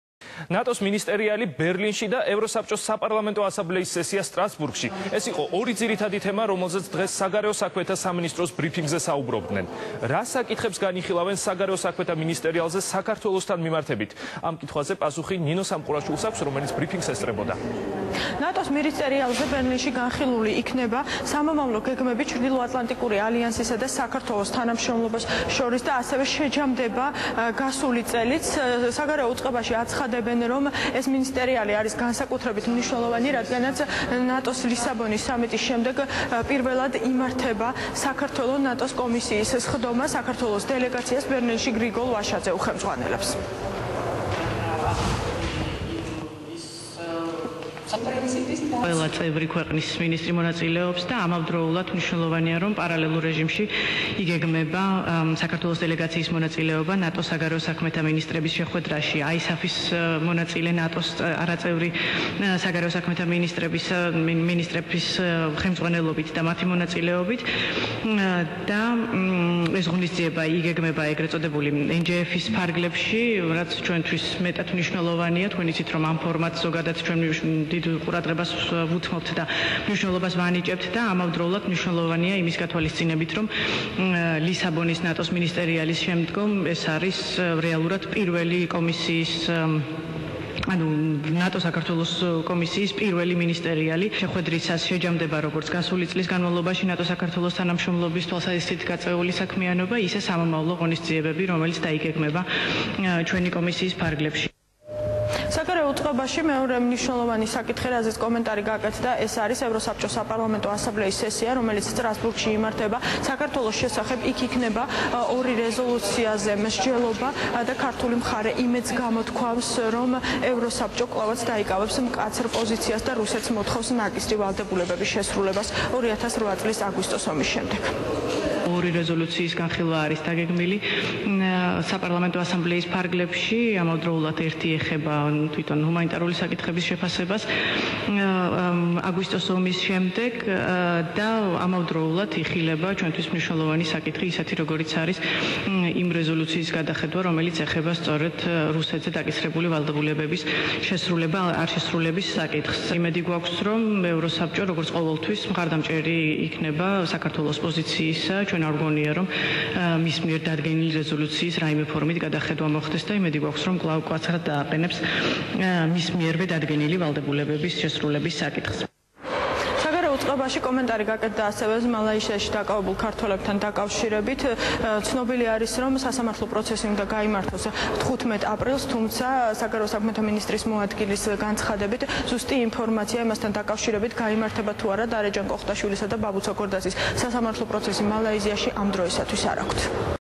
Briefing at the Ministry of Foreign Affairs
11.04.11

Deputy Minister of Foreign Affairs Nino Kalandadze held a traditional Monday briefing at the Ministry of Foreign Affairs today, voicing official positions regarding the upcoming NATO Ministerial in Berlin and the PACE session in Strasburg, where the issues of Georgia will be also discussed.

Nino Kalandadze said the Berlin Summit of Foreign Ministers was very important for Georgia, because the NATO-Georgia commission session would be held for the first time after the Lisbon summit. The commission will agree action plans for the future and sum up the activities of Georgian government carried out last year. Georgian delegation will be chaired by Minister Grigol Vashadze.

Nino Kalandadze also touched the upcoming session of the Council of Europe`s Parliamentary Assembly, where two resolutions will be discuss in regards with Georgia. Georgian side hopes that the Council of Europe will be as critical and sharp in its demands towards Russia to fulfill its obligations taken by previous resolutions, as before.

`One resolution refers to the assessment of humanitarian issues, the other will revise the previous resolutions, which obligates Russia to fulfill the terms of international agreements,` Nino Kalandadze said.